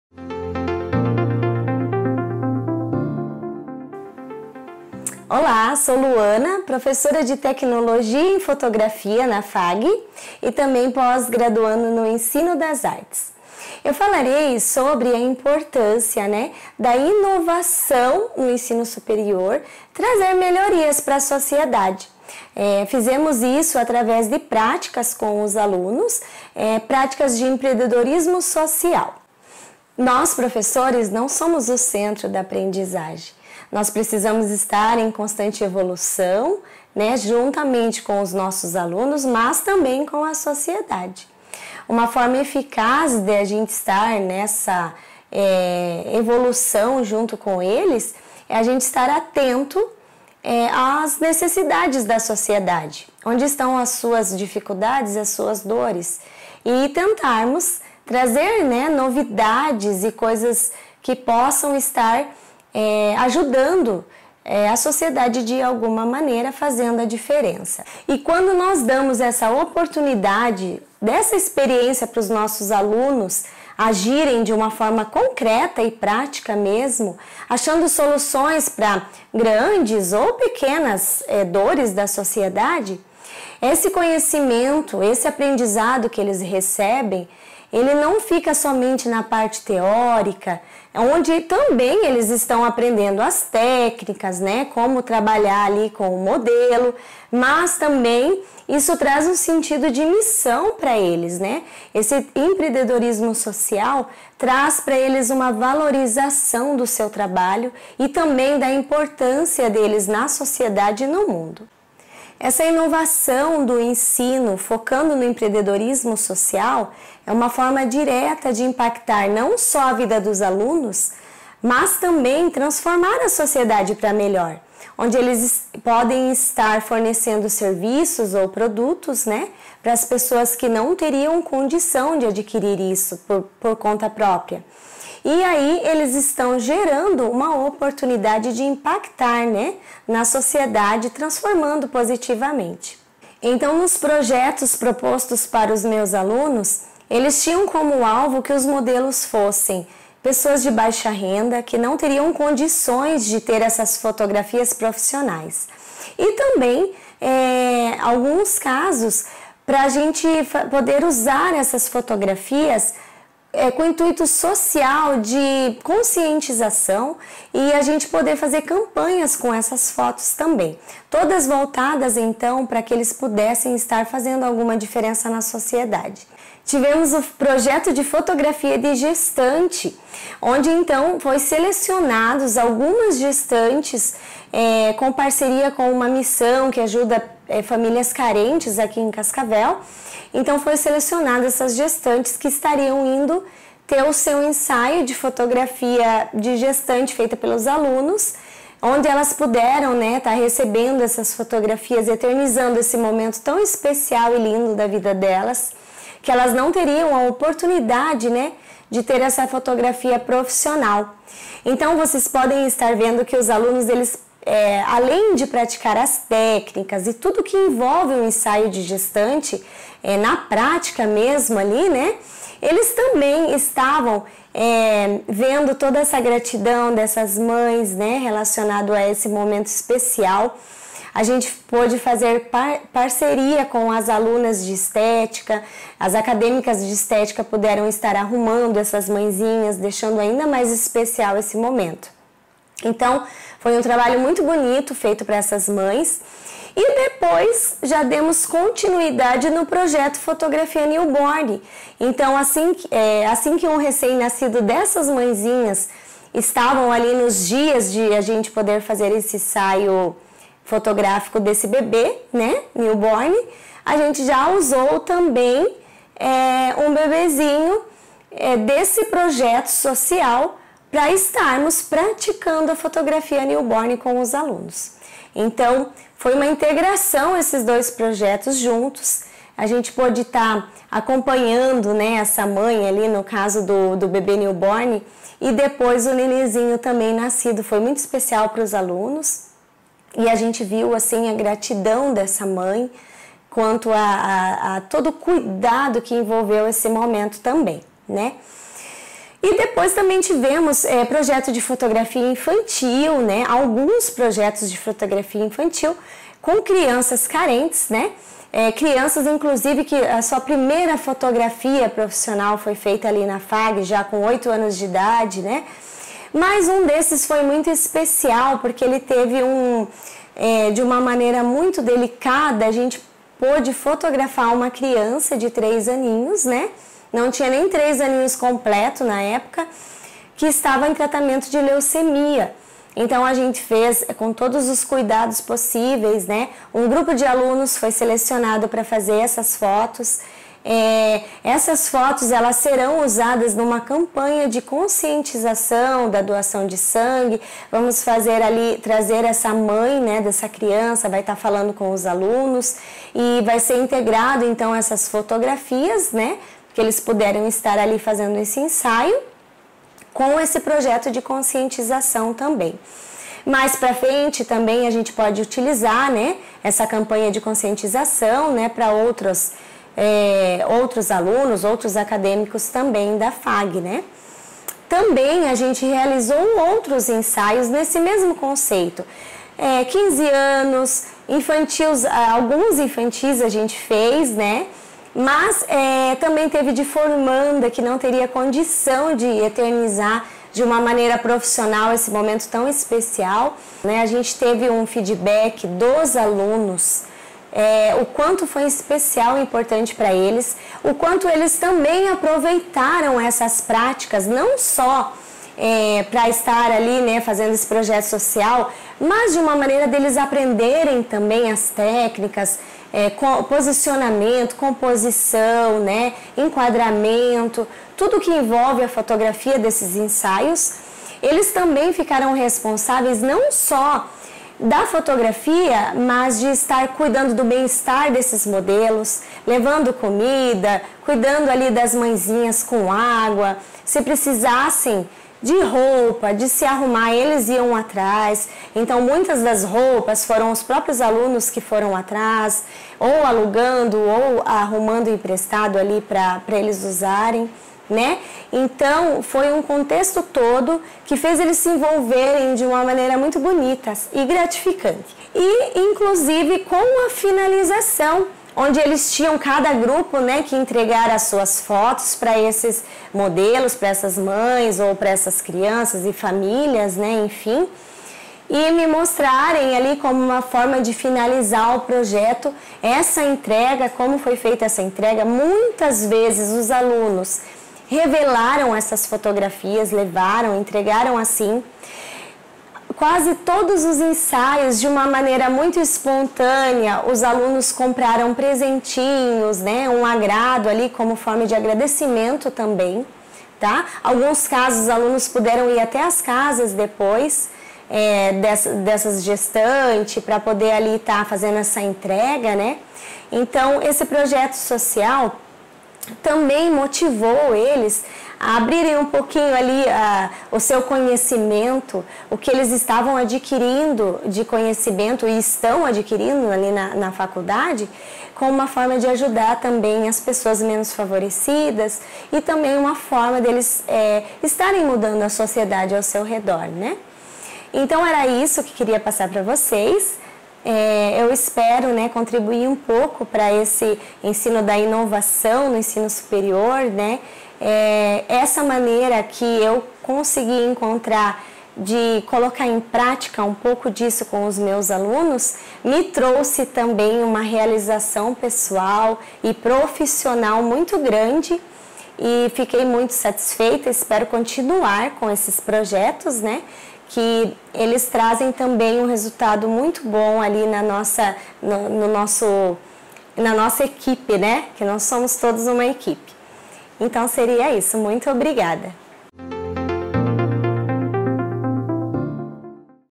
INOVAÇÃO FOCADA NO EMPREENDEDORISMO SOCIAL
Descrição: Neste relato a Professora do colegiado de Fotografia, Luana Strapasson ressalta que os professores devem constantemente estar conectados com a realidade social e de seus alunos e atentos às suas necessidades e dores. Os projetos mostrados neste vídeo são exemplos de inovação no ensino superior, criando oportunidades para os alunos colocarem em prática o empreendedorismo social, gerando serviços com impactos positivos, capazes de reduzirem estas problemáticas.